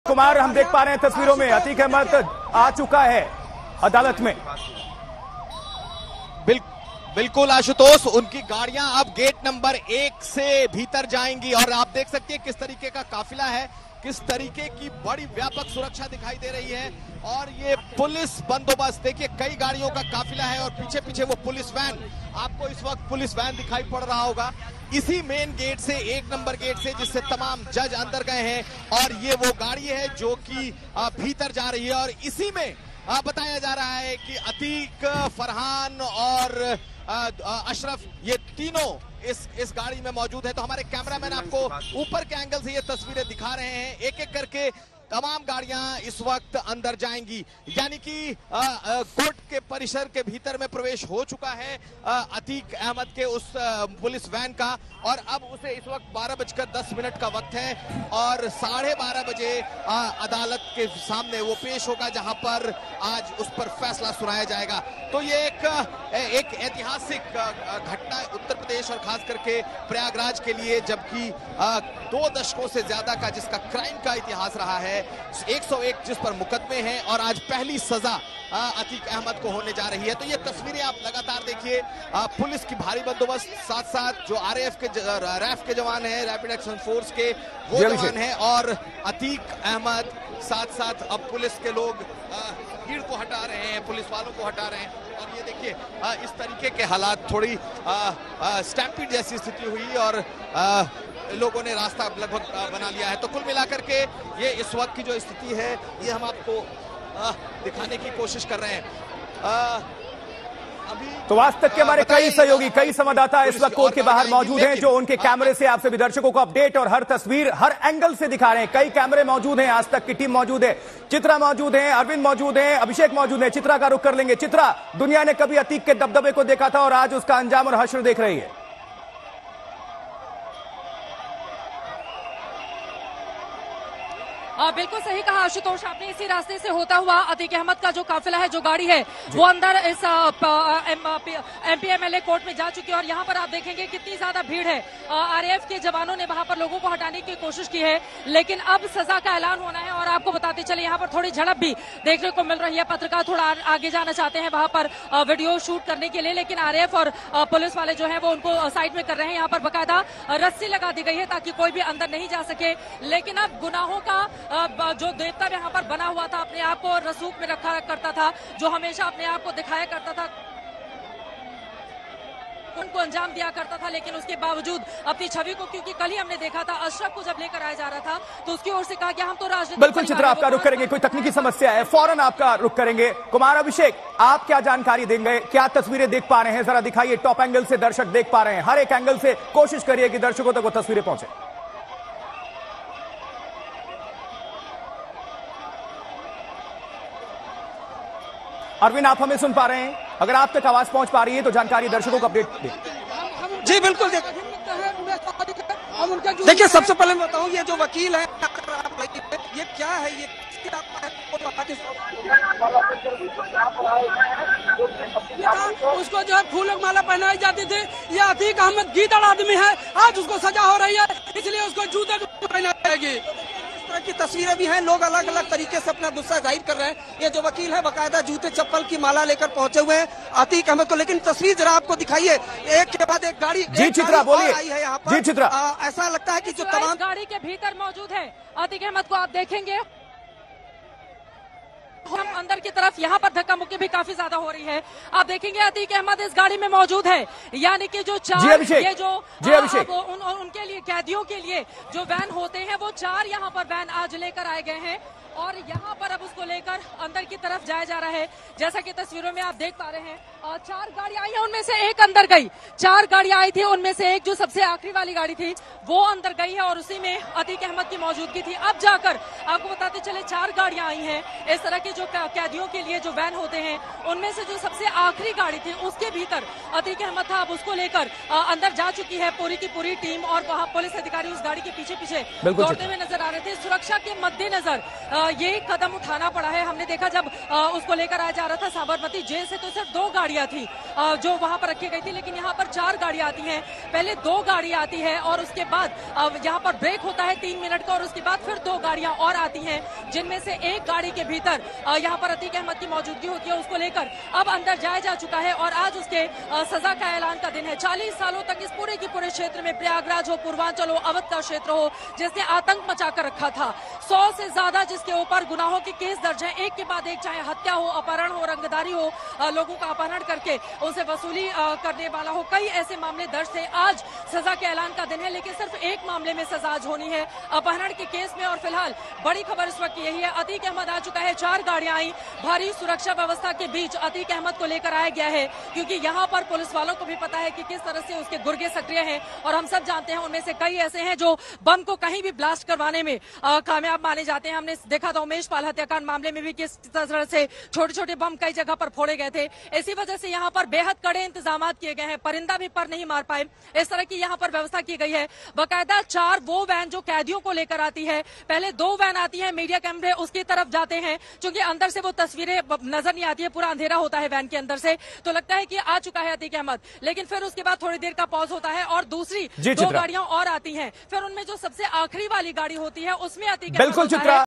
Breaking News: अदालत में अपराधियों की पेशी | Atique Ahmed | Umesh Pal | AajTak HD
25 जनवरी 2005 को बसपा विधायक राजू पाल की गोली मारकर हत्या कर दी गई थी. इस केस में अतीक अहमद, उसका भाई अशरफ समेत 5 आरोपी नामजद थे. जबकि चार अज्ञात को आरोपी बनाया था. इस केस में राजू पाल के रिश्तेदार उमेश पाल मुख्य गवाह था. उमेश का 28 फरवरी 2006 अपहरण हुआ था. इसका आरोप अतीक अहमद और उसके साथियों पर लगा था. उमेश ने आरोप लगाया था कि अतीक ने उसके साथ मारपीट की और जान से मारने की धमकी दी.

On 25 January 2005, BSP MLA Raju Pal was shot dead. In this case, 5 accused including Atiq Ahmed, his brother Ashraf were named. While four unknown were made accused. Raju Pal's relative Umesh Pal was the main witness in this case. Umesh was abducted on 28 February 2006. The charge was leveled against Atiq Ahmed and his associates. Umesh had alleged that Atiq assaulted him and threatened to kill him.